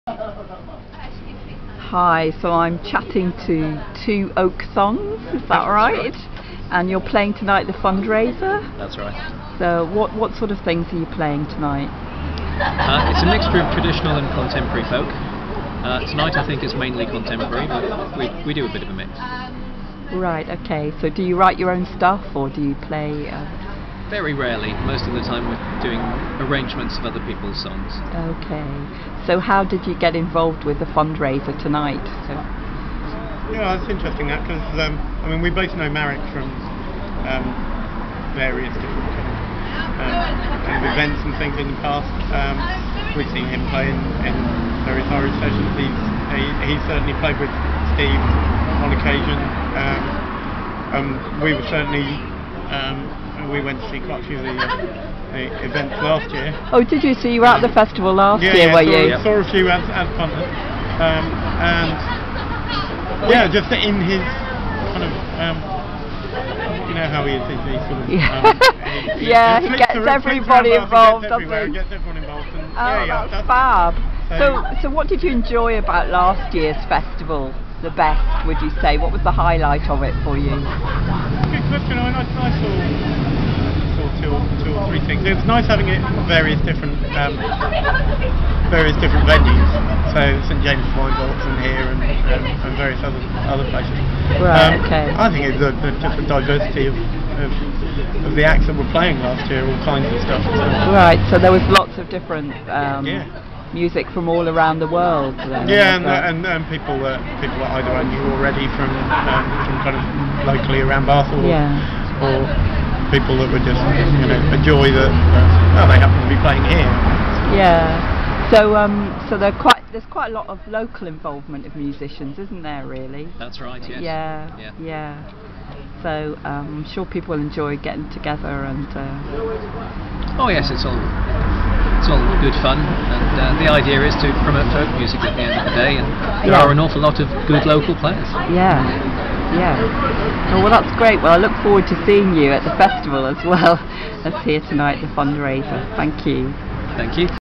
Two Oak Sons
Talking at Bath Folk Festival Fundraiser at Green Park Tavern on 20th July 2011